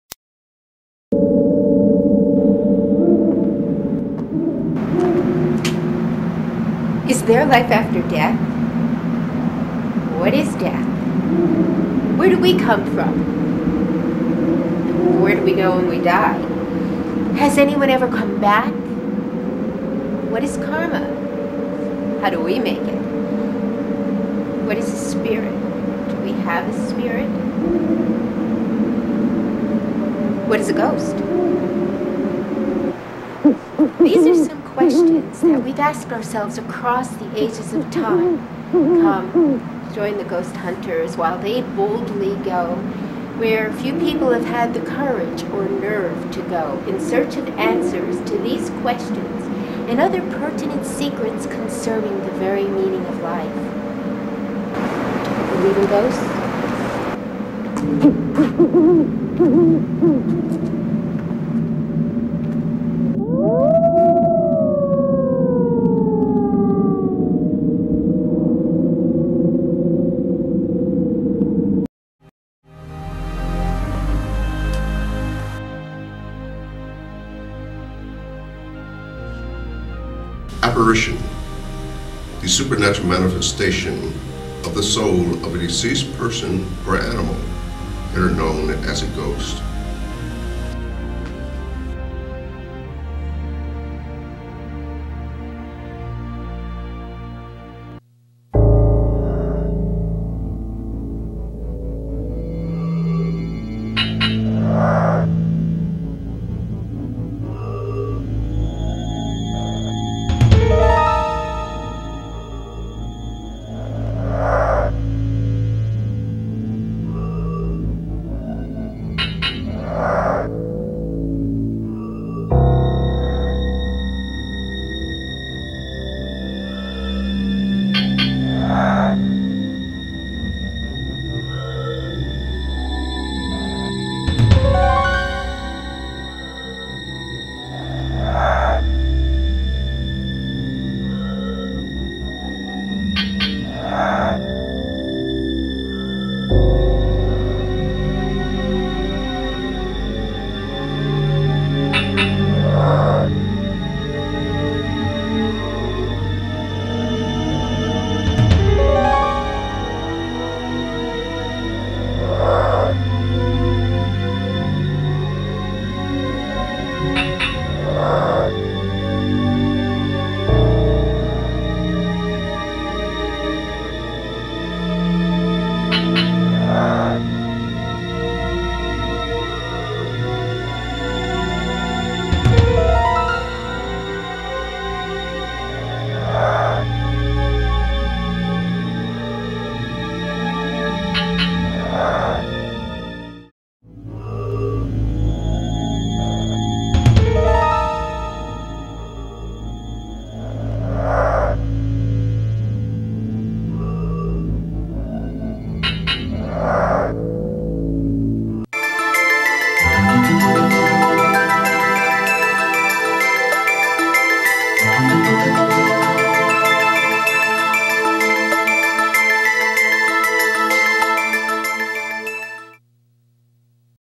Have Ghost Will Travel Troy Bookstore Ghost Around About Books
"Have Ghost-Will Travel" investigate the Around About Books Store in Troy Ohio